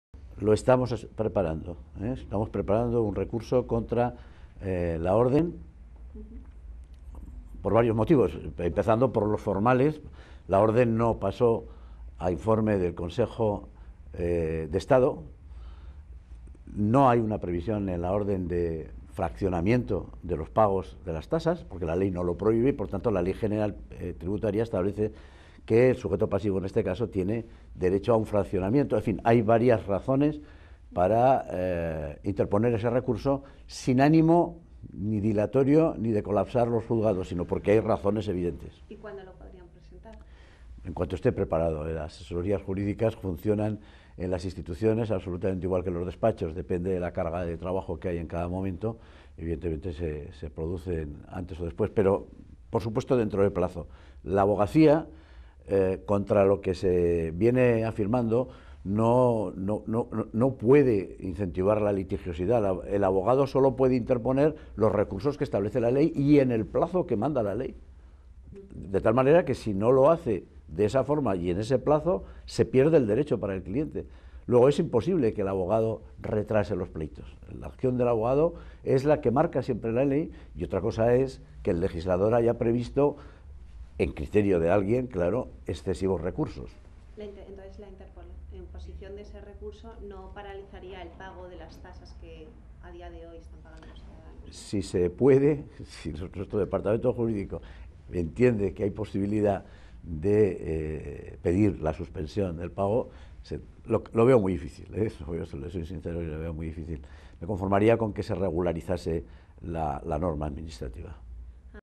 Carnicer: "Recurriremos ante la AN la ley de tasas"
El presidente del  Consejo General de la Abogacía Española (CGAE), Carlos Carnicer, ha afirmado que recurrirán ante la Audiencia Nacional la orden del Ministerio de Hacienda por la que se hizo efectivo el pago de las polémicas tasas judiciales al detectar una serie de defectos, como es la falta de previsión del fraccionamiento del pago o la ausencia del preceptivo informe del Consejo de Estado.